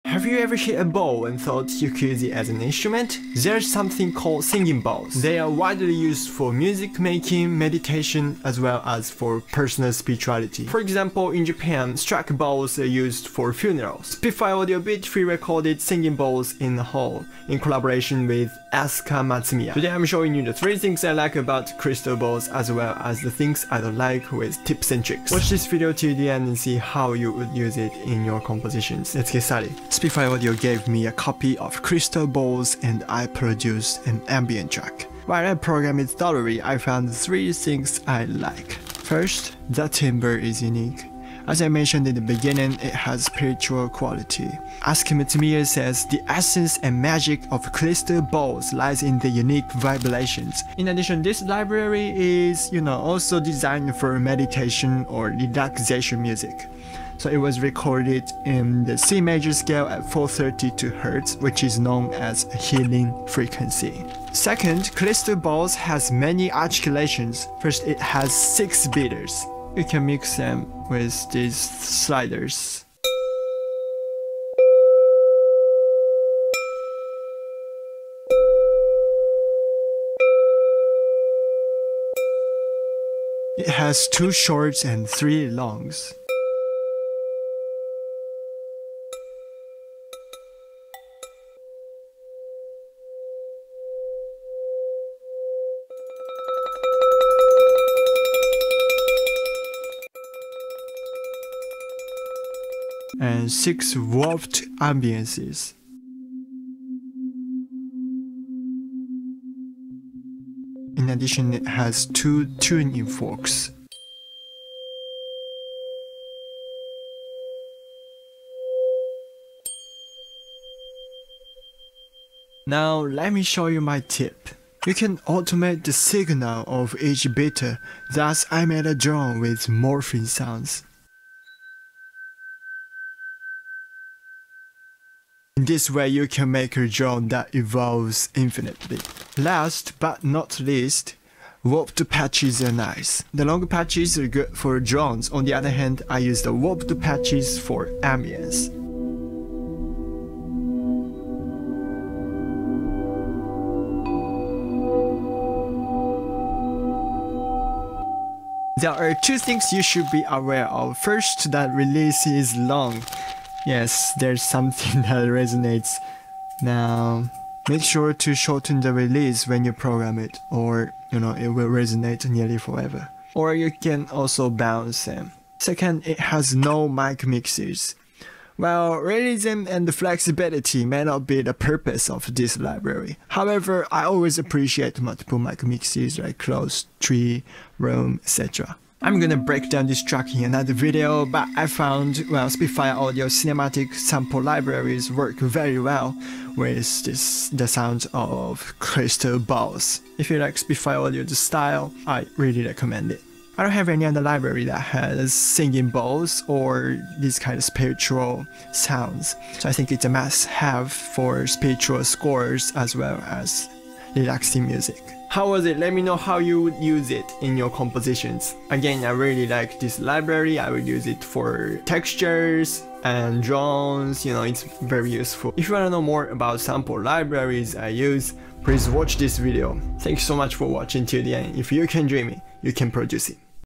Best VST for Relaxing Music?! | Spitfire Audio's Crystal Bowls Review
@spitfireaudiollp recorded singing bowls in collaboration with Aska Matsumiya.
Let's see how unique the library is!

Singing bowls are used for meditation as well as music making.
They have spiritual timbre.
Spitfire Audio recorded it at 432 Hz, the healing frequency.
I will use it for spiritual scores and relaxing music😁

*Spitfire Audio gave me a copy of Crystal Bowls

日本語字幕をつけました
日本の方々にも届くと嬉しいです！
画面上のCC ボタンからお楽しみください

===TIME STAMPS===
0:00 Intro
0:39 Unique Timbre
1:04 Articulations
2:03 Tip 1
2:19 Warped Patches
2:38 Things you should know with Tip 2
3:08 Do you need it?
3:35 More Sample Libraries!